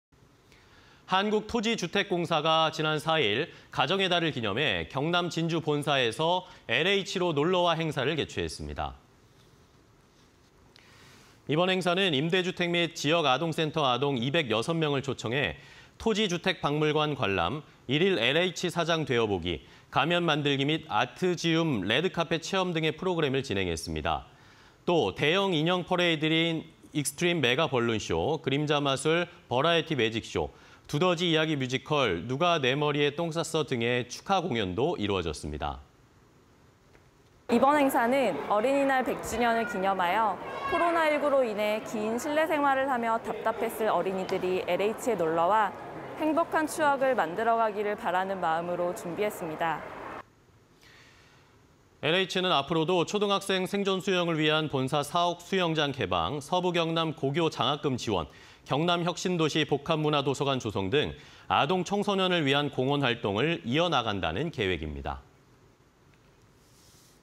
"LH 놀러와" LH, 어린이날 기념 아동 200여명 본사 초청 (20220506)
한국토지주택공사가 지난 4일 가정의 달을 기념해 경남 진주 본사에서 'LH로 놀러와' 행사를 개최했습니다.
이번 행사는 임대주택 및 지역 아동센터 아동 206명을 초청해 Δ토지주택박물관 관람 Δ일일 LH사장 되어보기 Δ가면 만들기 및 아트지움 레드카펫 체험 등의 프로그램을 진행했습니다.